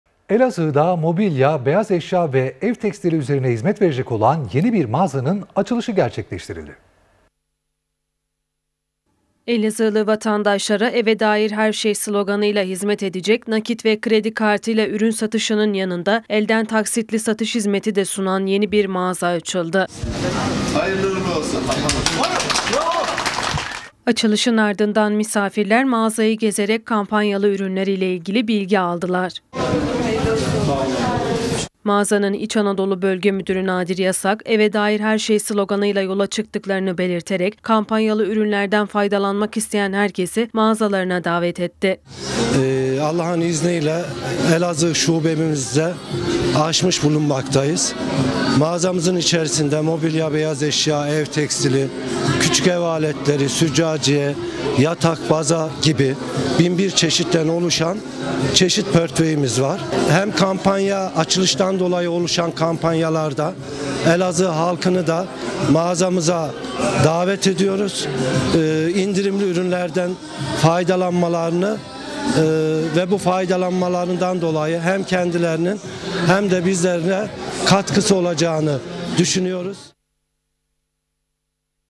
ELAZIĞ’DA ELDEN TAKSİTLİ SATIŞ YAPAN YENİ BİR MAĞAZA AÇILDI
ELAZIĞ’DA MOBİLYA, BEYAZ EŞYA VE EV TEKSTİLİ ÜZERİNE HİZMET VERECEK OLAN YENİ BİR MAĞAZANIN AÇILIŞI GERÇEKLEŞTİRİLDİ